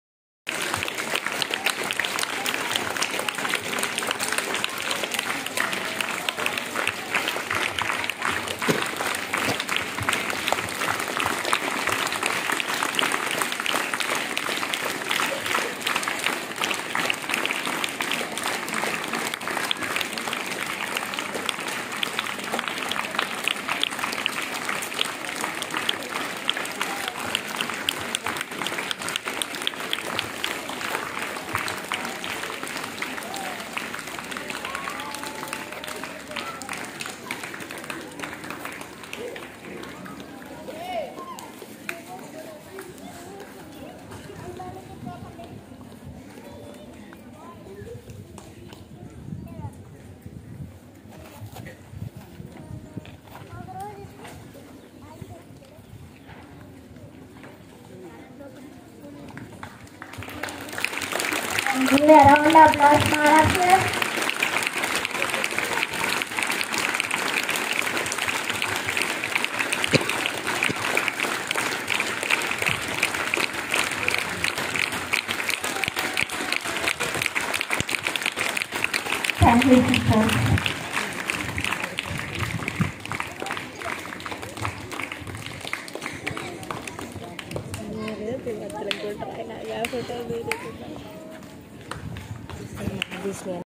November 9, 2022
St. Anne's English Medium school
pedakurapadu
Realising Educational Activities Broucher by Sr. Antony